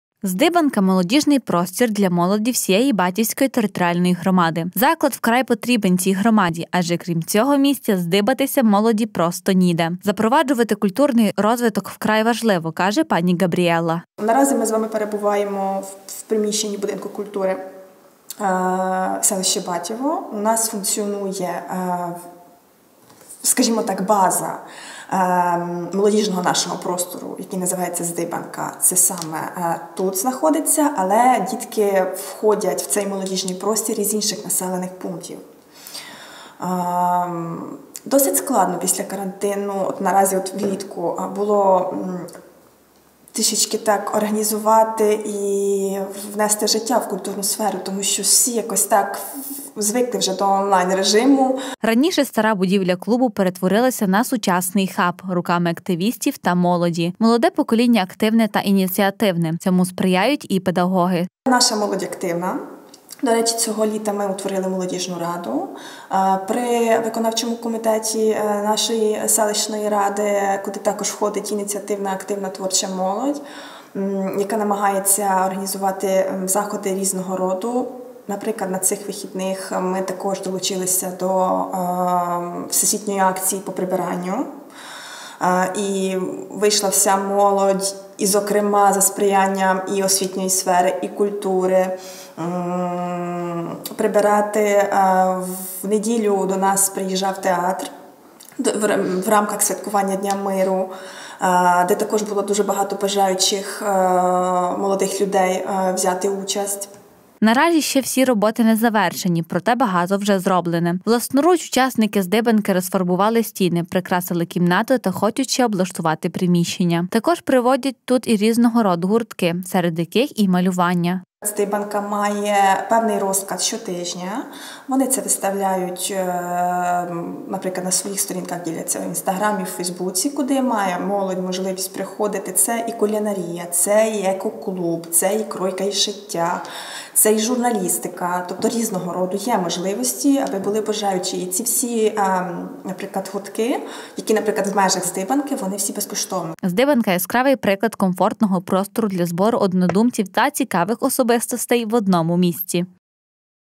«Здибанка» - простір для молоді
У Батьові на території селищної ради функціонує культурний центр «Здибанка». Тут збирається молодь з усієї громади та беруть участь у різних проектах та громадських ініціатив. «Здибанка» - молодіжний простір для молоді всієї Батівської територіальної громади.  Заклад вкрай потрібен цій території , адже крім цього місця збиратися молоді просто ніде. Запроваджувати культурний розвиток вкрай важливо, каже пані Габріелла. Раніше стара будівля клубу перетворилася на сучасний ХАБ руками активістів та молоді. Молоде покоління активне та ініціативне. Цьому сприяють і педагоги. Наразі ще всі роботи не завершені, проте багато вже зроблене. Власноруч учасники «Здибанки» розфарбували стіни, прикрасили кімнату та хочуть ще краще облаштувати приміщення.  Також проводять тут і різного роду гуртки, серед яких і малювання.  «Здибанка» - яскравий приклад комфортного  простору  для збору однодумців та цікавих особистостей в одному місті.